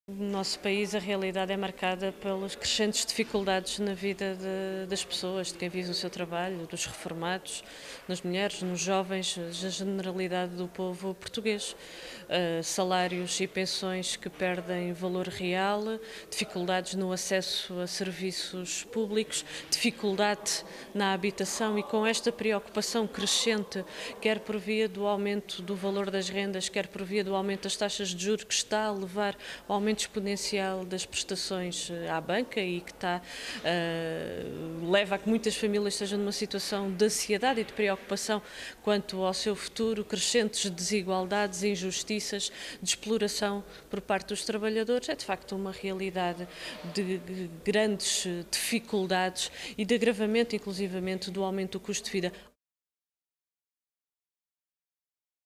PCP alerta para falta de resposta a “crescentes dificuldades”
A líder parlamentar do PCP considera que a atual situação nacional é de “crescentes dificuldades” e injustiças na sociedade portuguesa, acusando o PS de se “recusar a dar as respostas que são necessárias” e defendendo uma “política alternativa”.

Edição de Franque Silva